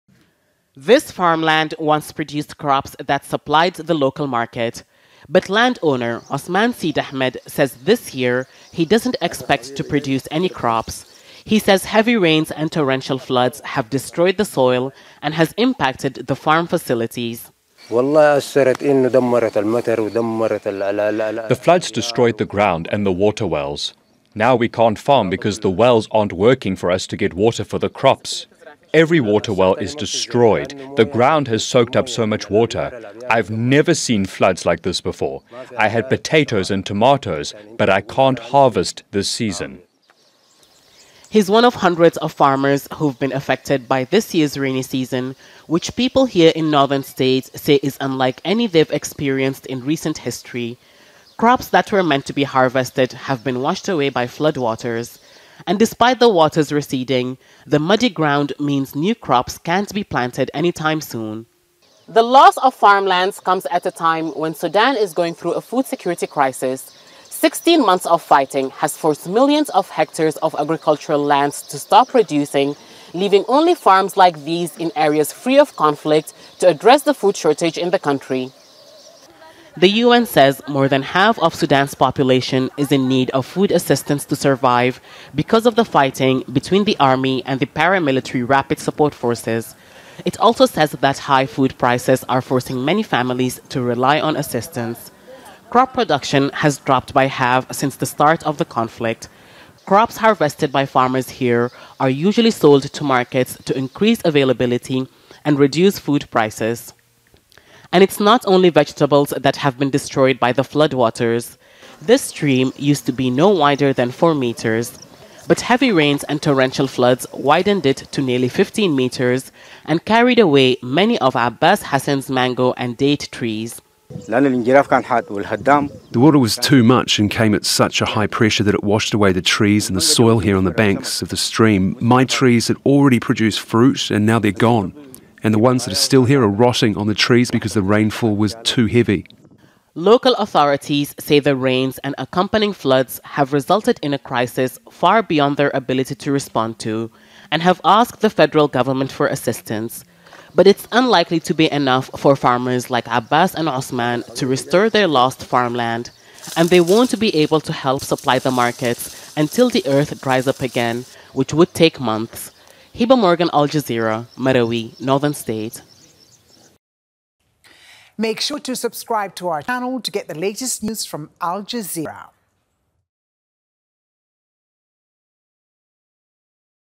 Heavy floods destroy farms in Sudan: Ruined crops worsen food crisis
More than half of Sudan's population is living under the threat of hunger after flooding devastated large areas of farmland.
Months of conflict between the Army and the paramilitary Rapid Support Forces have also hampered food production.
Severe rains are worsening the situation.

Al Jazeera’s Hiba Morgan reports from Sudan’s Northern state.